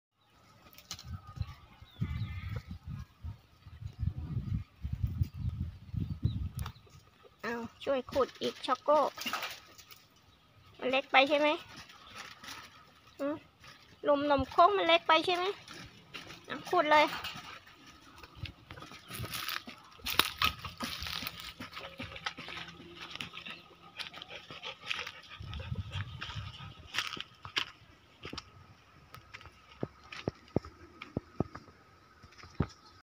หมาเฒ่า เจ้าช๊อคโก้ จอมขยัน
อยากช่วย... ว่าส่าน ด้วยนิสัยทีชอบขุ้ยเขี่ย ลุยเลยคร้าาา